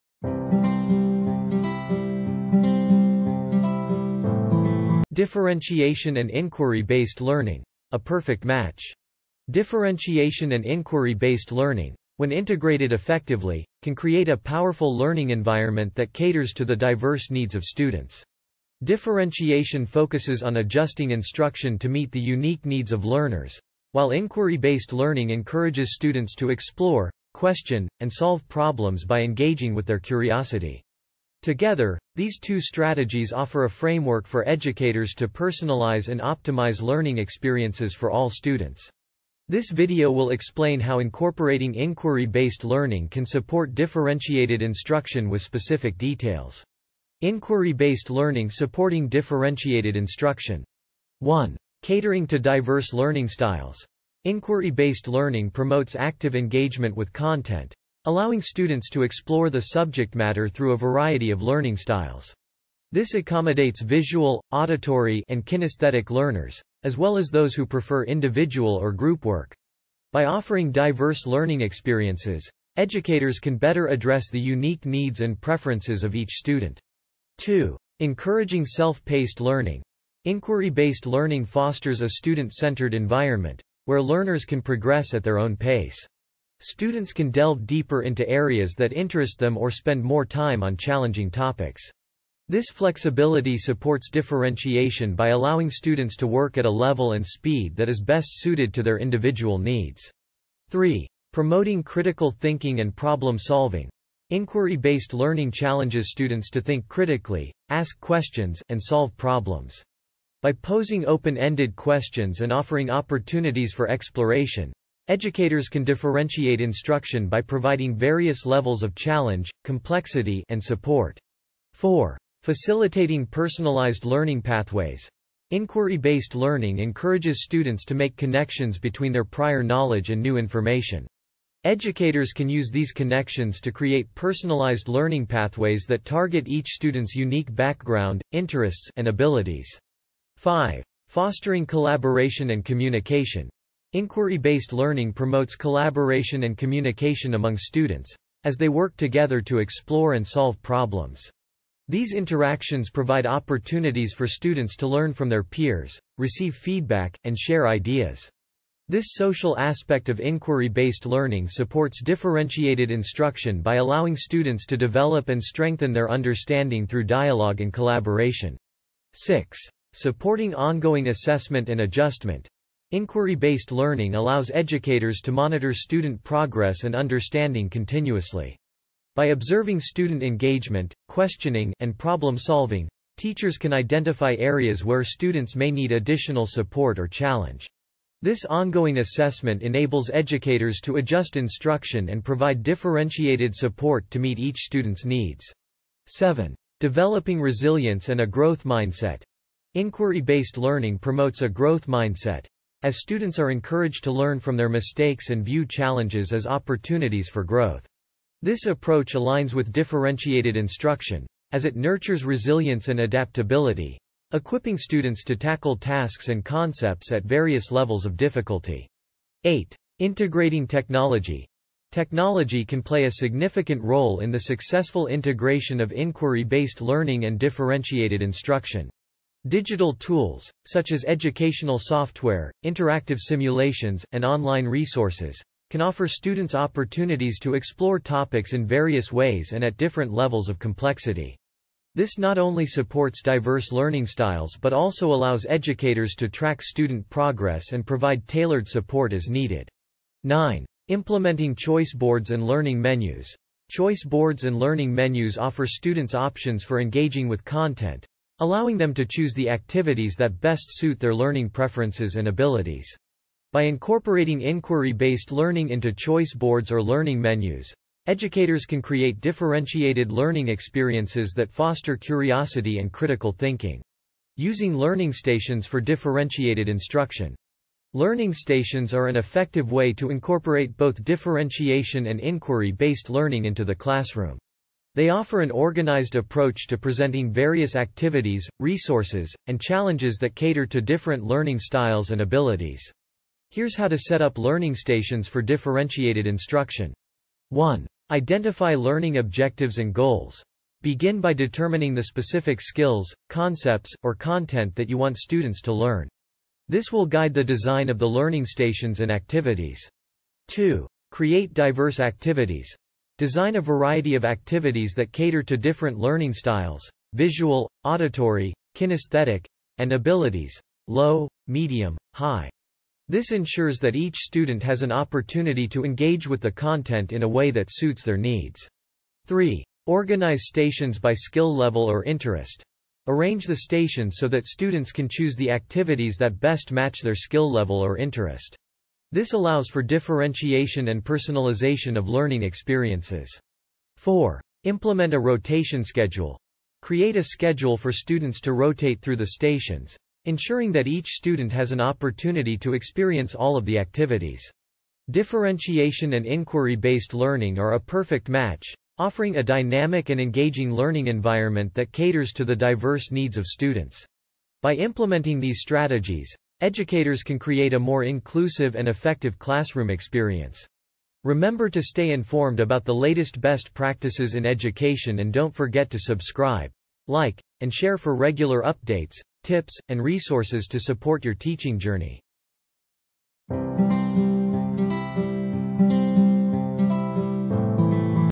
Differentiation & Inquiry-Based Learning: A Perfect Match
Differentiation and inquiry-based learning, when integrated effectively, can create a powerful learning environment that caters to the diverse needs of students. Differentiation focuses on adjusting instruction to meet the unique needs of learners, while inquiry-based learning encourages students to explore, question, and solve problems by engaging with their curiosity.
Together, these two strategies offer a framework for educators to personalize and optimize learning experiences for all students.
This video will explain how incorporating inquiry-based learning can support differentiated instruction with specific details.